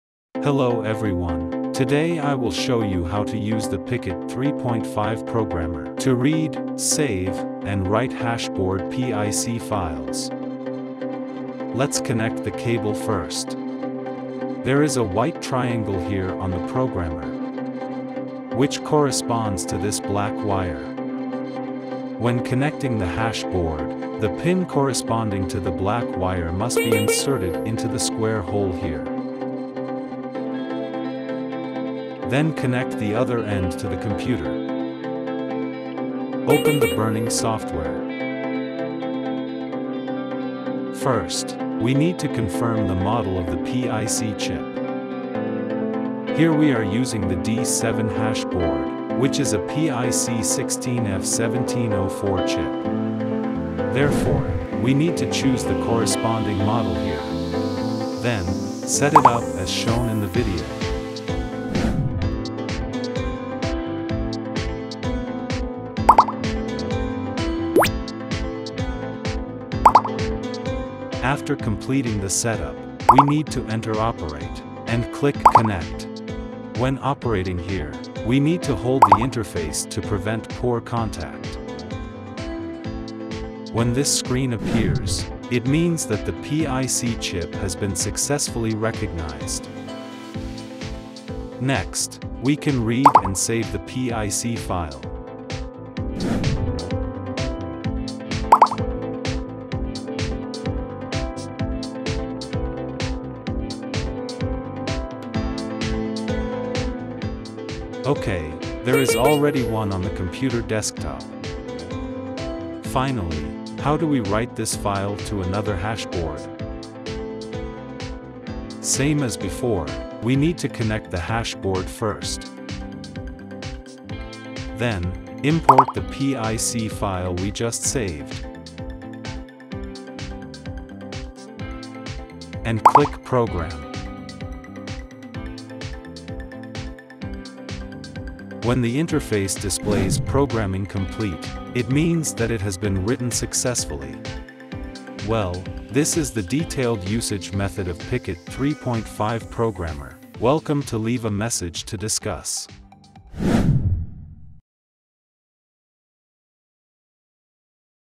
How to use PICKit 3.5 programmer to burn PIC file - a tutorial on reading, saving and writing steps
PICkit3.5 online debugging programmer is used to read, save and write hash board PIC chip data. If S19 S19pro S17+ S17e S17 T17+T17eT17 S15 T15 S11 S9se S9k S9i S9iS9T9+T9 Z9 Z9miniz15 S17Pro S19iS19+T19 S19iS19iPro S19a D7 and other antminer series hashboards cannot work properly due to PIC chip data damage or loss, you can Use this PICKit 3.5 programmer to program with programming tools. Note: The miner needs the corresponding PIC file programming.

The tools used in this video are:
PICKit 3.5 programmer：

00:00 Introduction
00:12 Cable connection
00:36 Read and save PIC files
01:53 Write PIC file to another hash board